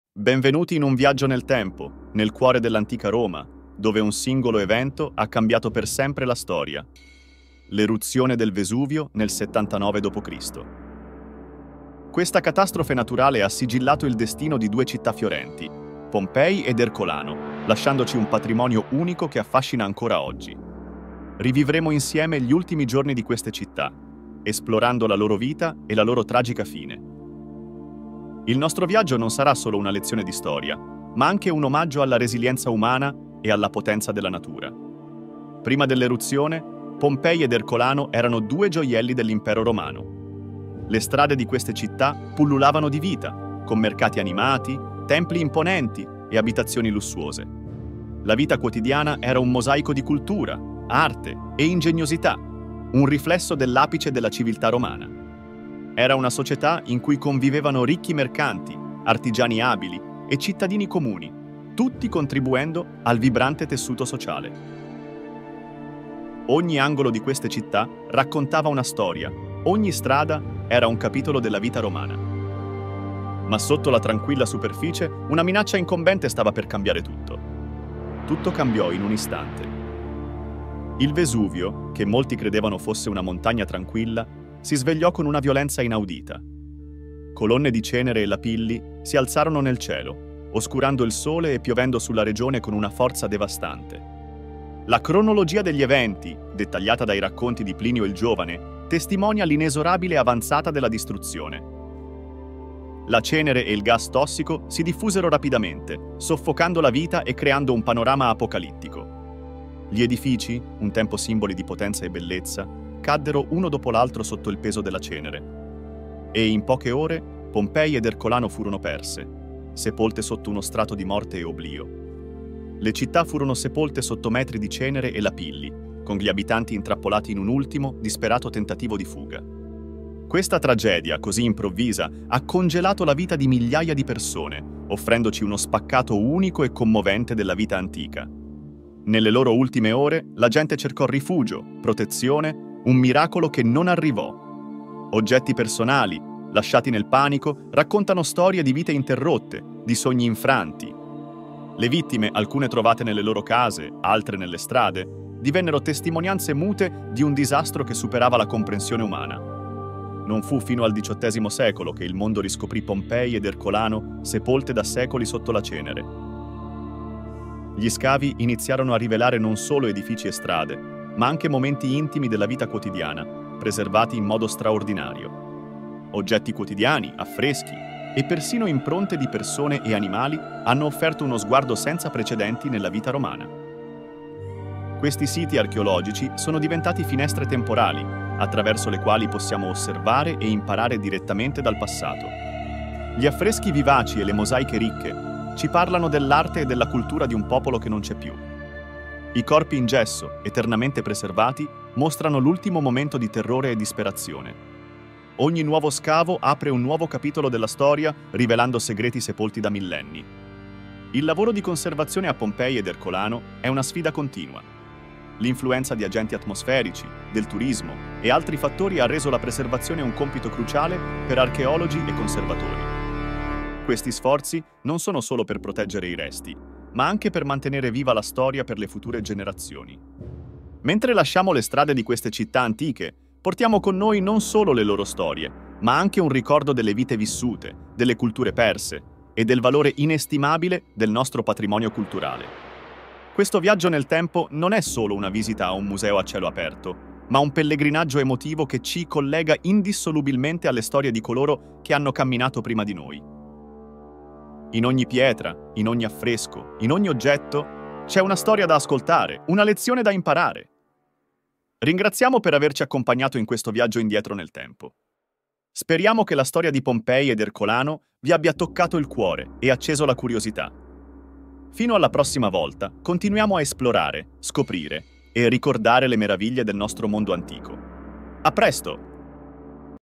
"Pompei: Vita Prima della Catastrofe" - Una finestra nel passato romano!
"Esplora con noi uno dei momenti più tragici e affascinanti della storia antica: l'eruzione del Vesuvio del 79 d.C. Questo video porta alla luce le storie nascoste sotto la cenere di Pompei ed Ercolano, città romane inghiottite da un destino fatale. Attraverso immagini mozzafiato e ricostruzioni storiche, scopriremo come la vita quotidiana si è trasformata in un dramma di sopravvivenza. Testimonianze archeologiche, interviste con esperti e una narrazione avvincente ci guideranno in un viaggio indietro nel tempo, rivelando come questa catastrofe abbia plasmato la storia e lasciato un segno indelebile sulla nostra comprensione del mondo antico. Unisciti a noi in questo viaggio emozionante attraverso la storia e la scienza, mentre sveliamo i segreti sepolti dalla cenere vulcanica."